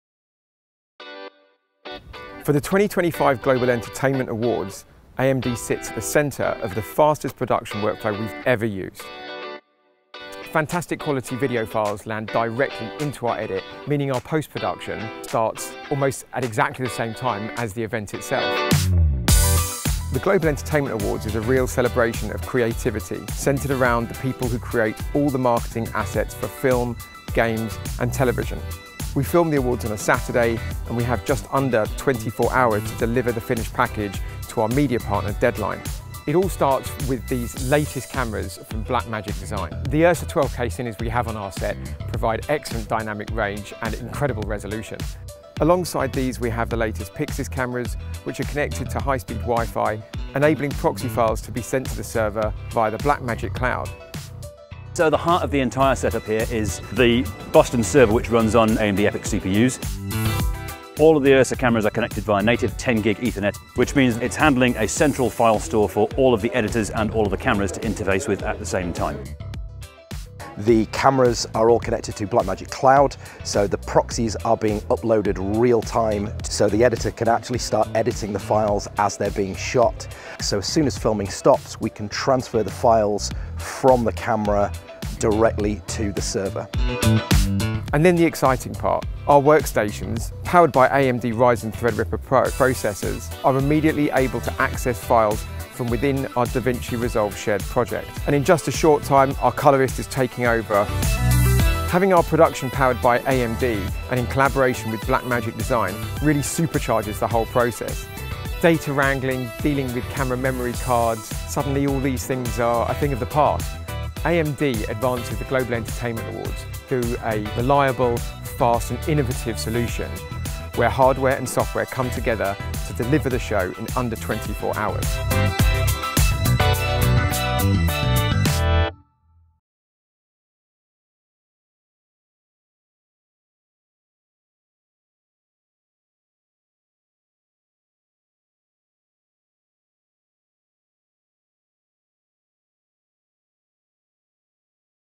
How AMD & Blackmagic Delivered a 24-Hour Post Workflow | Global Entertainment Awards 2025
See how AMD and Blackmagic Design powered the fastest post-production workflow ever used for the Global Entertainment Awards. From shooting to delivery in under 24 hours, this behind-the-scenes look reveals how DaVinci Resolve, running on AMD EPYC™ and Ryzen™ Threadripper™ CPUs, handled real-time editing with Blackmagic URSA 12K and PYXIS cameras, all connected via high-speed cloud and 10Gb Ethernet.

Discover how:
• Files hit the edit bay almost as filming ends
• Editors collaborate in real-time via Blackmagic Cloud
• AMD hardware eliminates traditional bottlenecks
• The final package is delivered in less than a day

Whether you're a post-production pro, systems integrator, or creative technologist, this is a glimpse into the future of live event production.